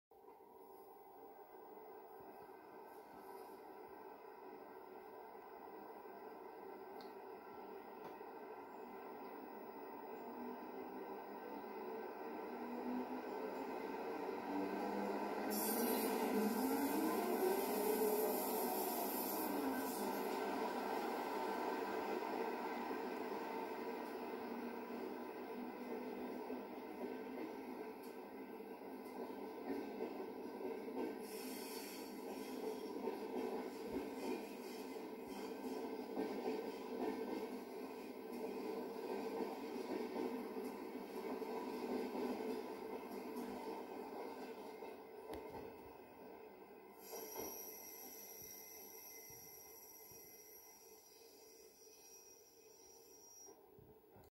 RETRO-low quality: NS 1145 vertrekt slippend uit Amersfoort met een InterRegio trein, 06-06-1998
Niet de beste kwaliteit, de opname is (nog) niet gedigitaliseerd maar simpelweg gefilmd vanaf een TV scherm. Niet echt geschikt voor de fijnproevers maar wel voor hen die herinneringen willen ophalen aan vervlogen tijden op het spoor van de vorige eeuw.